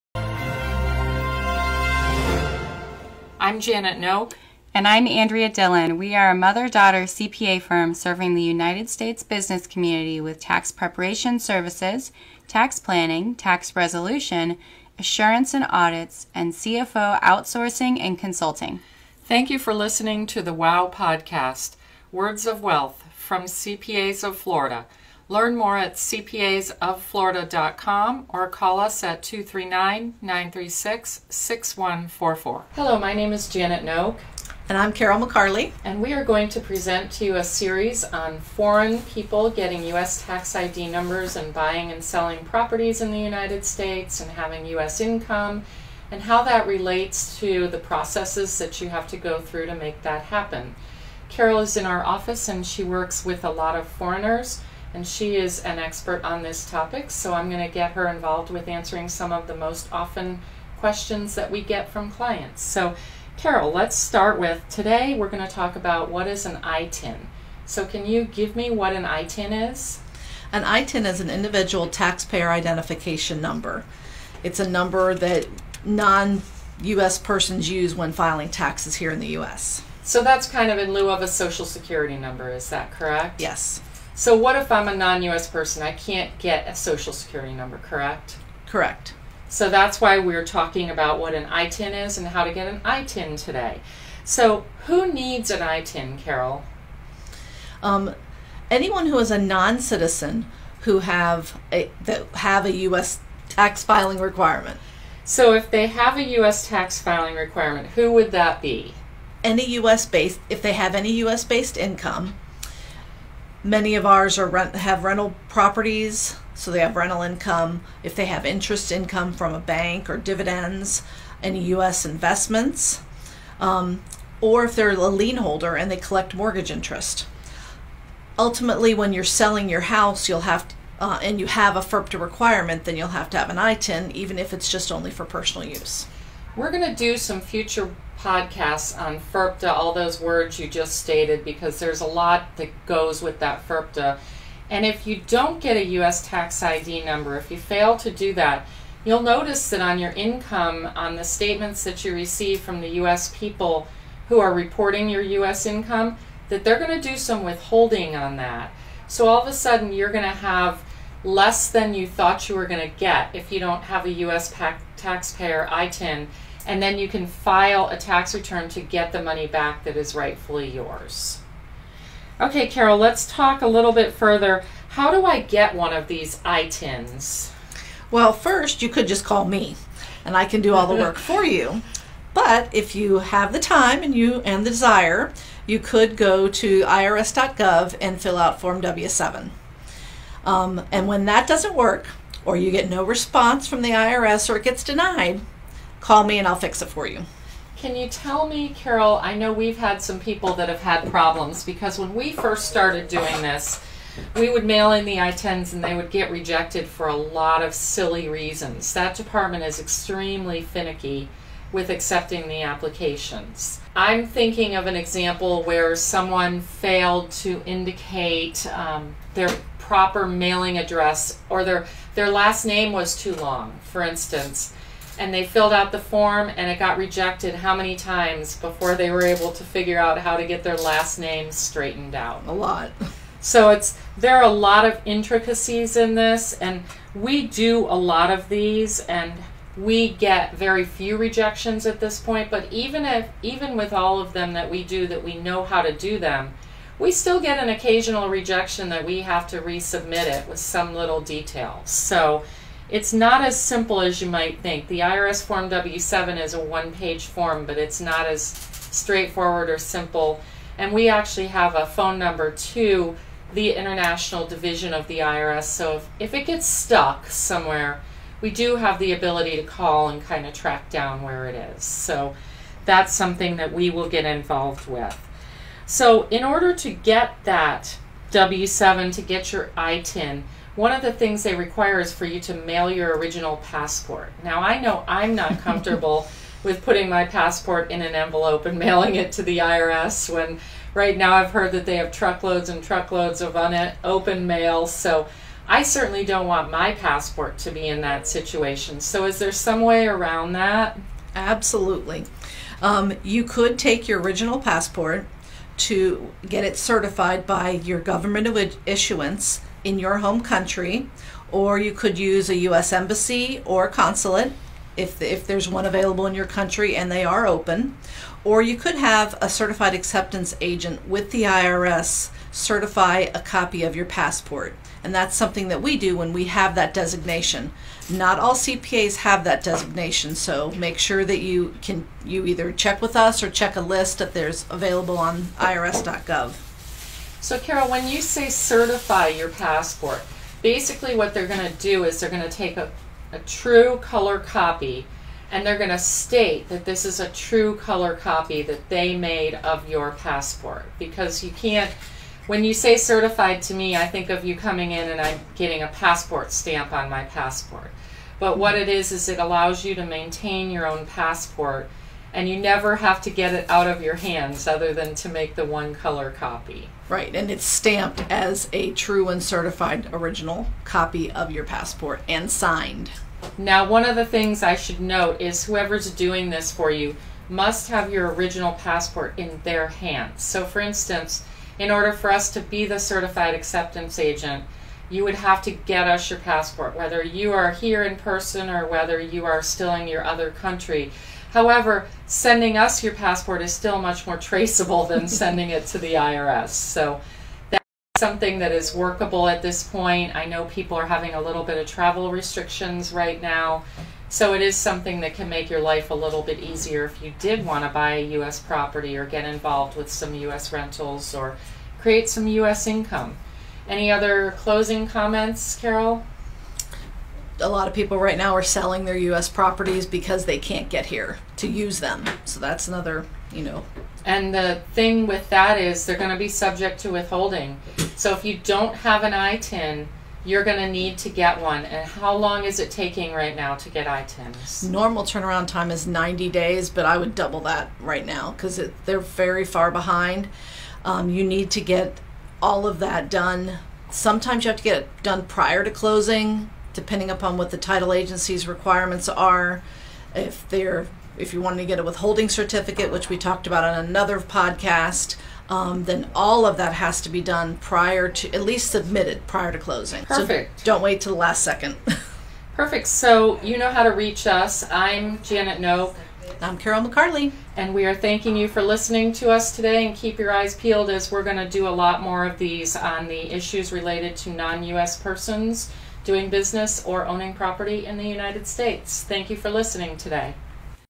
𝗔𝘀 𝗮 𝗡𝗼𝗻-𝗨.𝗦 𝗣𝗲𝗿𝘀𝗼𝗻 𝗗𝗼 𝗜 𝗡𝗲𝗲𝗱 𝗔 𝗨.𝗦 𝗜𝗧𝗜𝗡
What is an ITIN? How can I get an ITIN? Why do I care? Learn why and what it takes to get a US ITIN. Join Carol McCarley and Janet Noack for a series on Non-US persons and US taxation.

Fort Myers Office 🏤
12610 World Plaza Lane Ste 1

Naples Office 🏤
5621 Strand Blvd Ste 305